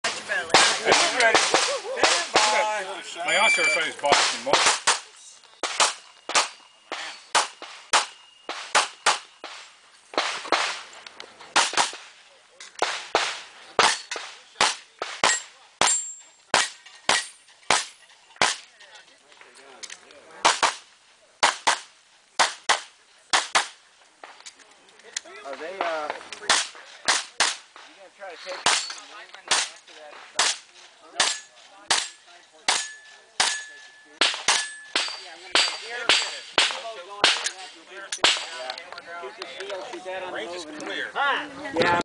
USPSA Stage 4 at PSC 8/17/13
Shot 6th in production division. Athena Lee from season 2 of Top Shot got 5th in production, she beat me by about 5%.